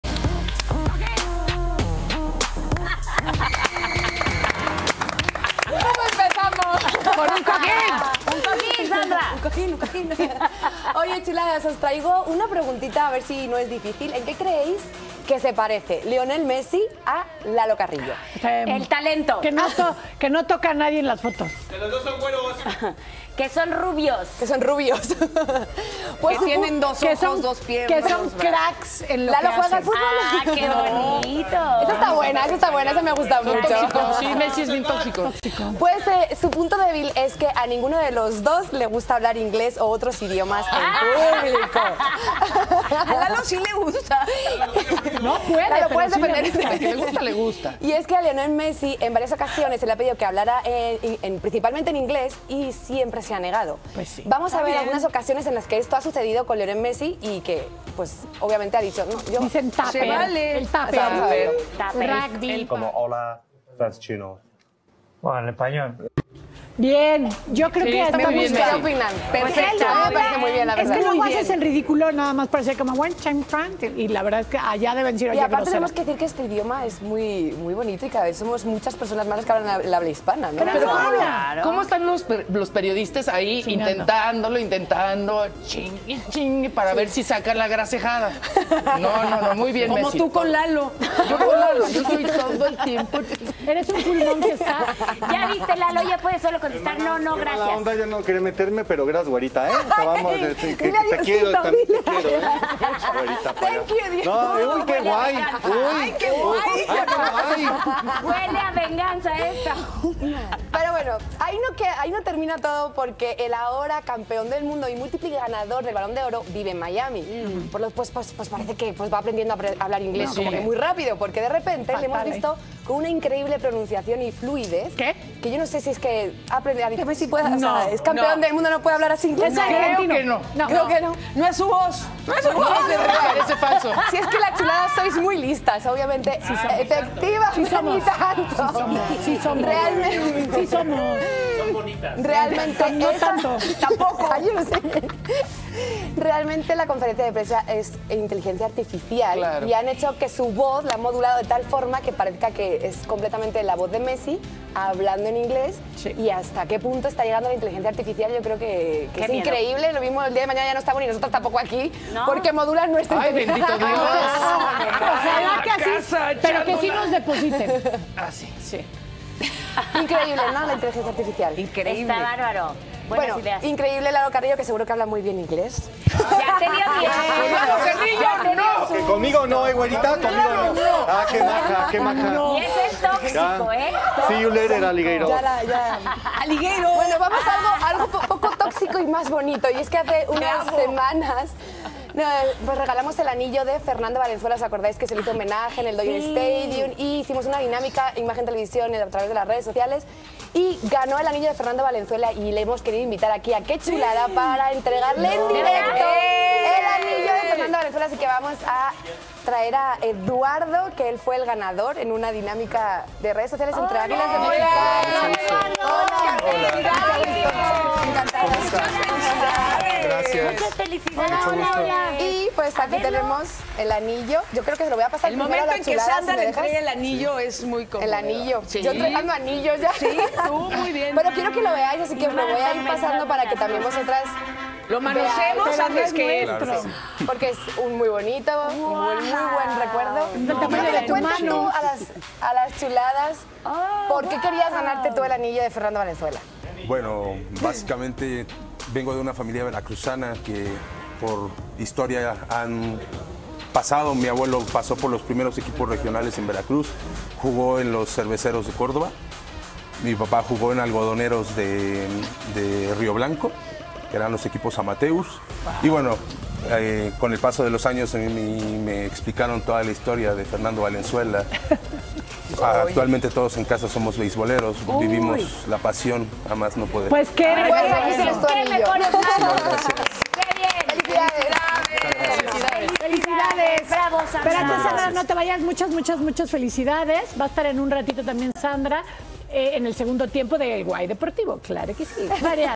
¡Messi se NIEGA a hablar en inglés! | ¡Qué Chulada!
Messi se NIEGA a hablar en inglés, no se le da muy bien el idioma 🤷‍♂️